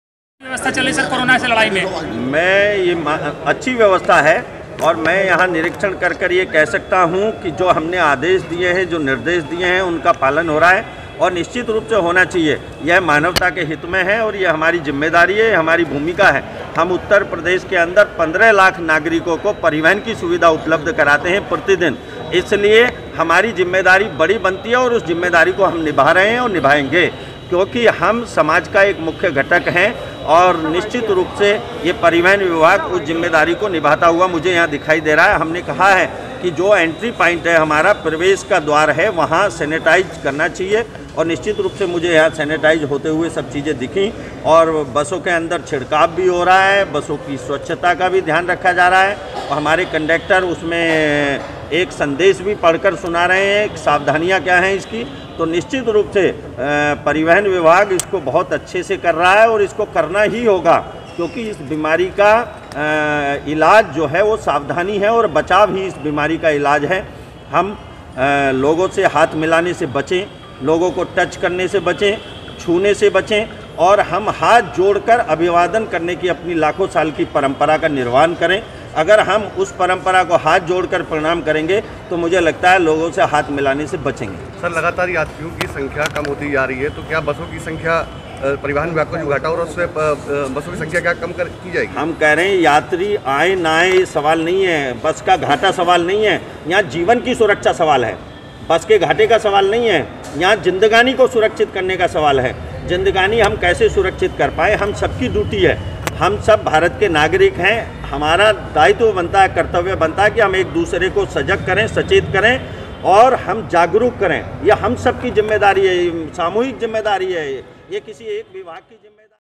परिवहन मंत्री पहुंचे निरीक्षण करने आलमबाग बस अड्डे
Video from Jai Hind